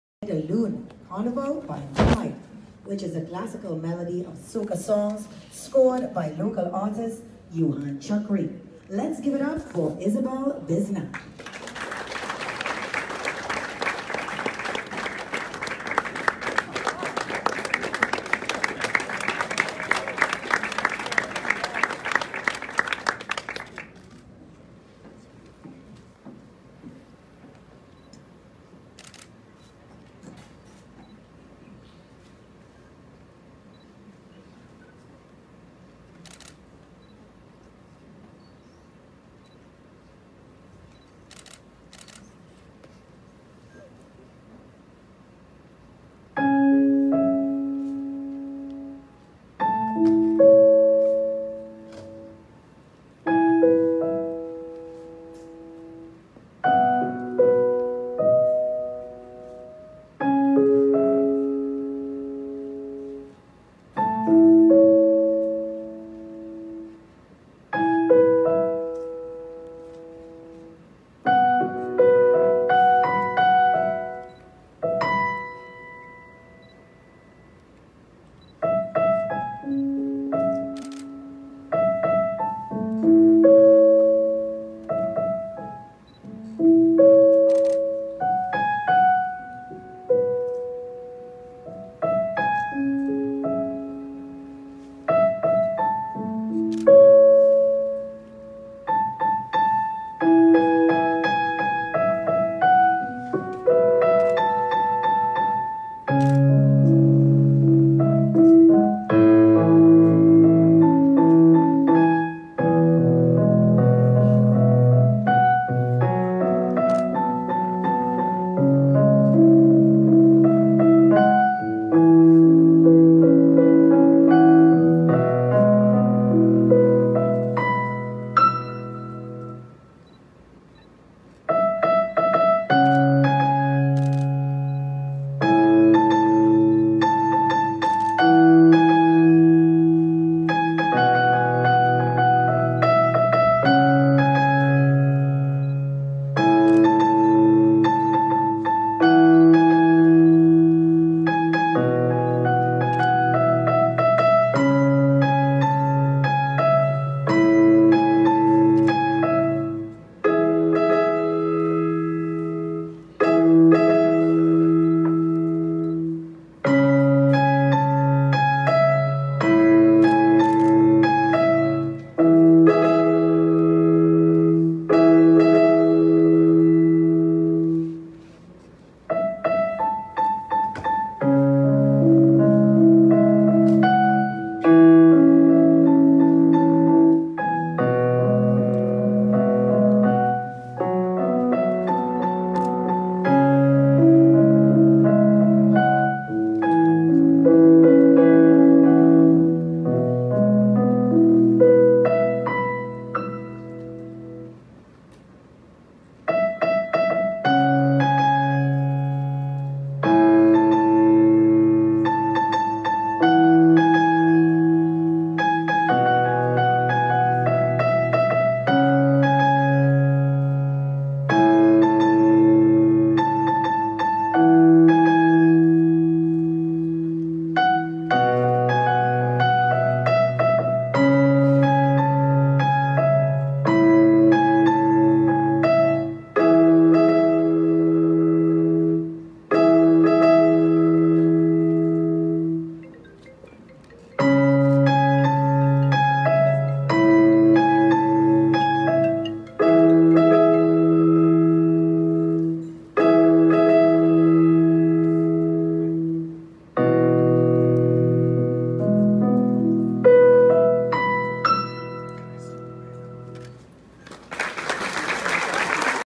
Brilliant Piece by World Class Pianist, Ysabel Bisnath
Ysabel Bisnath talent at Miss Trinidad and Tobago 2018 contest!